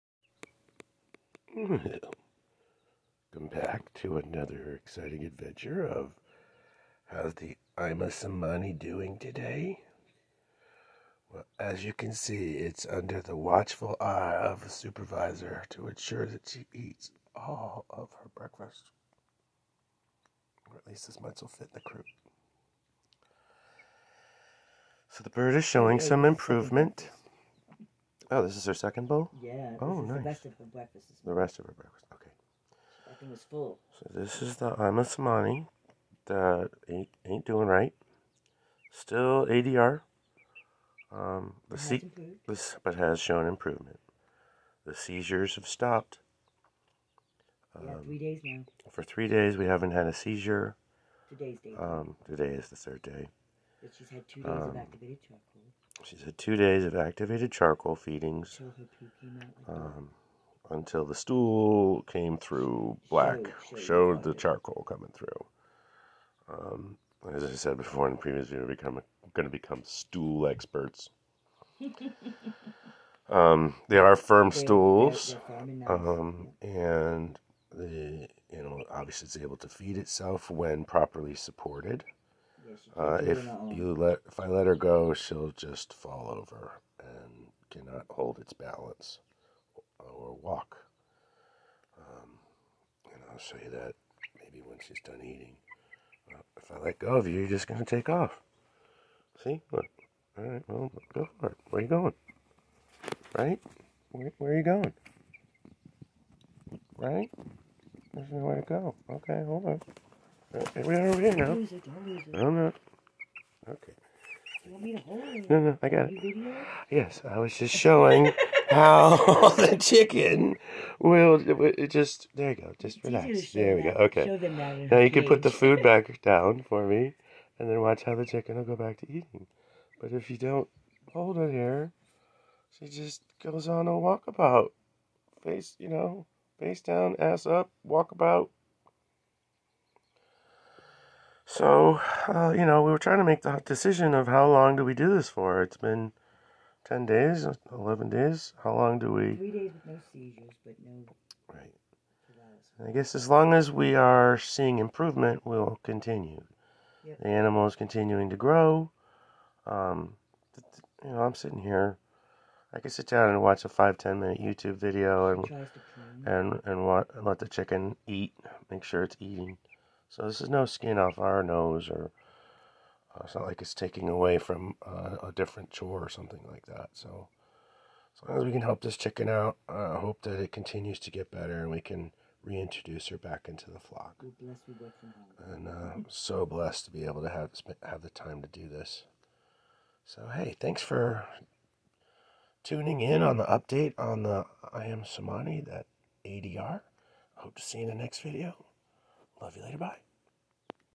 ADR Ayam Cemani 7 weeks old. Update January 3, 2024
We see improvement so we will continue to help this little chicken heal.  any advice and well wishes is greatly appreciated 😎